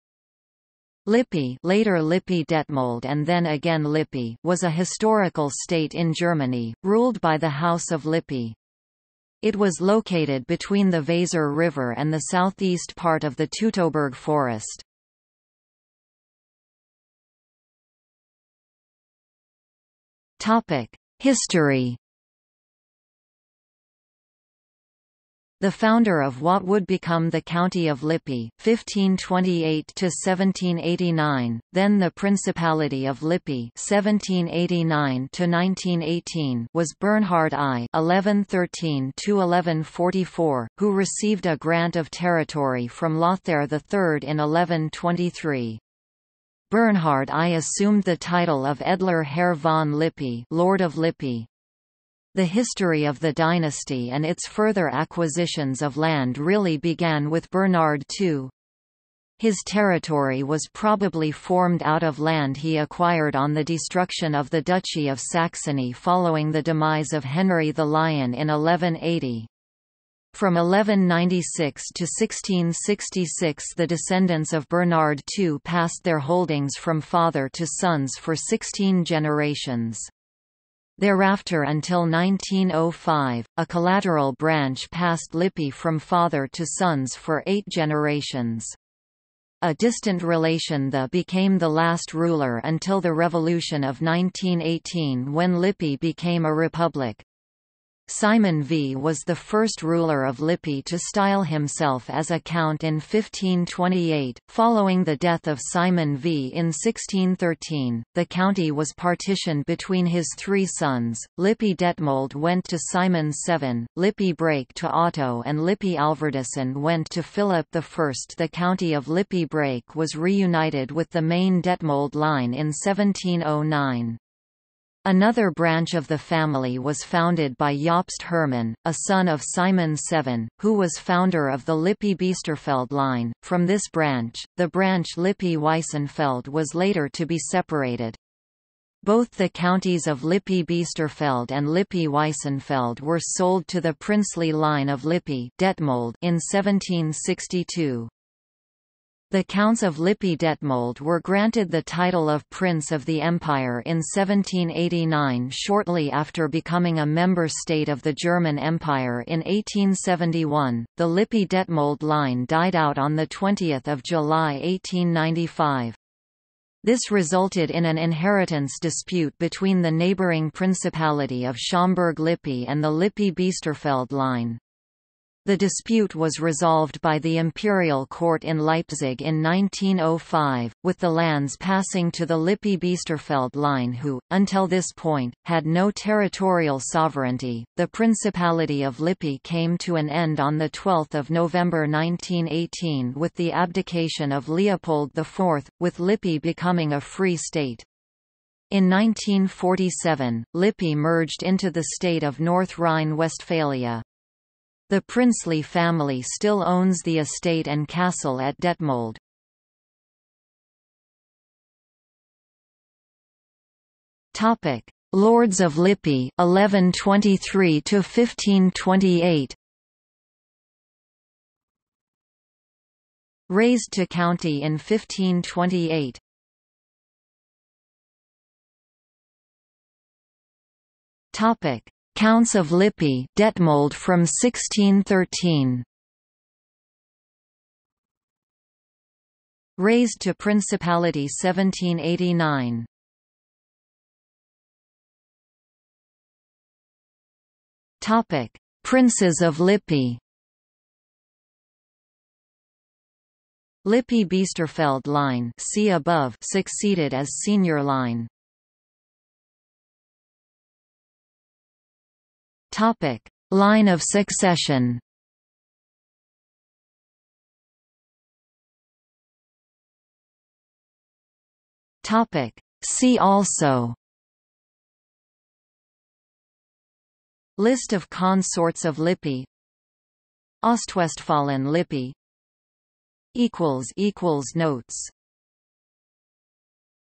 Principality of Lippe | Wikipedia audio article
00:00:16 1 History
00:03:30 2 Lords of Lippe (1123–1528)sup&#91;3&#93;/sup
00:03:47 3 Counts of Lippe (-Detmold from 1613)sup&#91;4&#93;/sup
00:04:04 4 Princes of Lippesup&#91;5&#93;/sup
00:04:19 4.1 Line of Successionsup&#91;6&#93;/sup
00:04:28 5 See also

- Socrates

SUMMARY
Lippe (later Lippe-Detmold and then again Lippe) was a historical state in Germany, ruled by the House of Lippe. It was located between the Weser River and the southeast part of the Teutoburg forest.